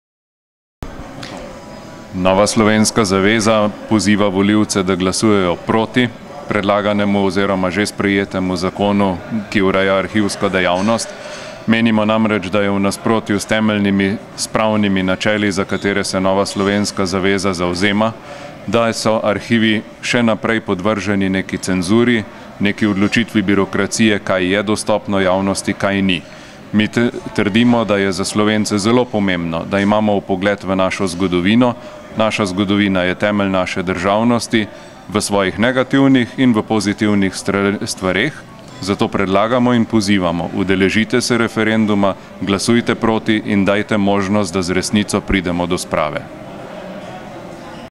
Peter Sušnik PROTI zapiranju arhivov, PROTI zanikanju resnice
Peter Sušnik, predsednik Nove Slovenske zaveze je povedal , zakaj so v NSZ proti zapiranju arhivov in proti zanikanju resnice. »Nova Slovenska zaveza poziva volilce, da glasujejo proti sprejeti noveli zakona, ki ureja arhivsko dejavnost.« Kot je povedal, v NSZ menijo, »da je novela v nasprotju s temeljnimi spravnimi načeli, za katere si NSZ zavzema.«

Po Sušnikovih besedah so arhivi podvrženi cenzuri in odločitvi birokracije, kaj je dostopno in kaj ne. »Naša zgodovina je temelj naše državnosti,« zato je pomembno, da Slovenci poznamo negativne in pozitivne elemente svoje sgodovine.

Predsednik NSZ Peter Sušnik je ob koncu volivce še pozval, naj 8. junija na referendumu izrečejo odločni PROTI. »Predlagamo in pozivamo, udeležite se referenduma, glasujte proti in dajte možnost, da z resnico pridemo do sprave.«